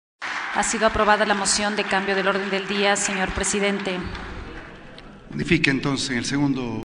Votación - Sesión 494 - Cambio del orden del día
10 de enero de 2018
Resolución, la comparecencia de @CarlosDLTorreEC para que aclare el retiro de 300 millones de la cuenta de Solidaridad que habrían sido utilizados en la liquidez del Estado